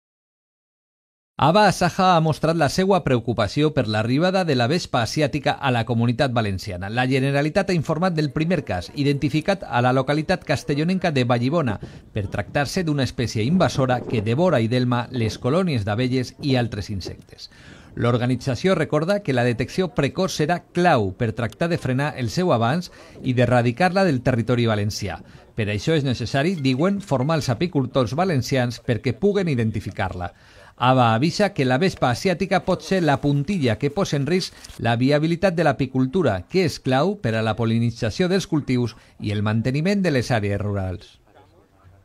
Grup Televisió - Confirmat el primer cas de vespa asiàtica en la Comunitat Valenciana
AVA-ASAJA mostra la seua “preocupació” per l'arribada de la vespa asiàtica a la Comunitat Valenciana